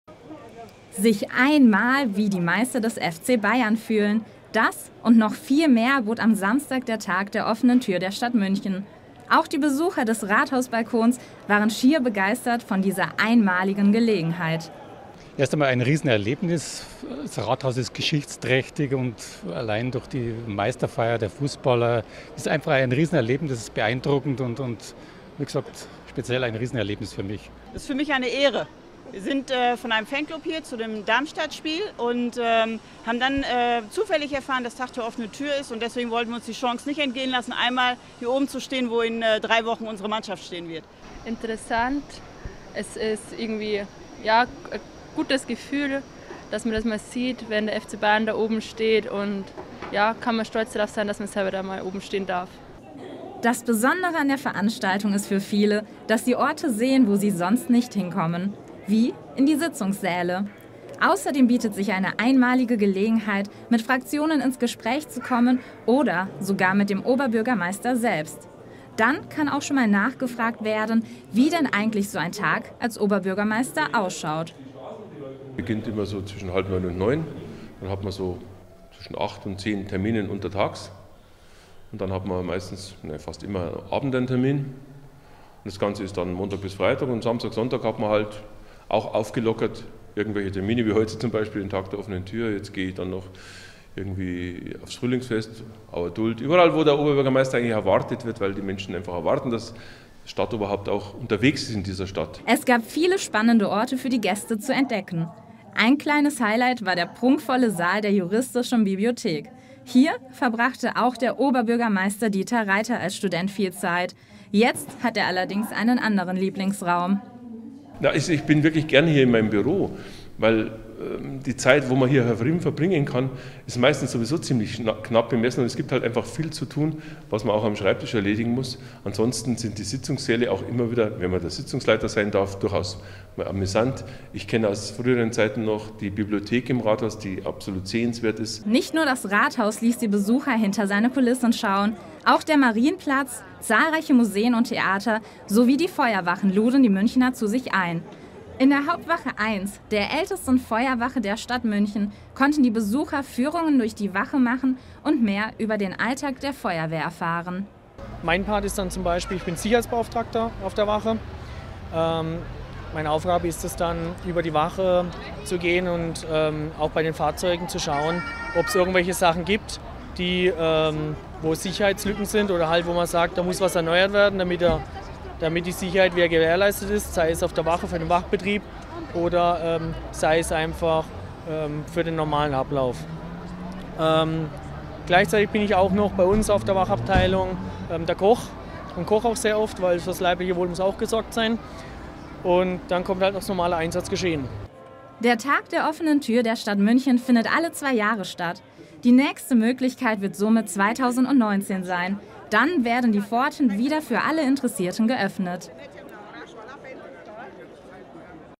"Sich einmal wie die Meister des FC Bayern fühlen"
Über 24.000 Besucher waren am "Tag der offenen Tür" der Stadt München unterwegs um einmal hinter die Kulissen zu schauen. Nicht nur das Rathaus lud zu sich ein - auch zahlreiche Museen, Theater und …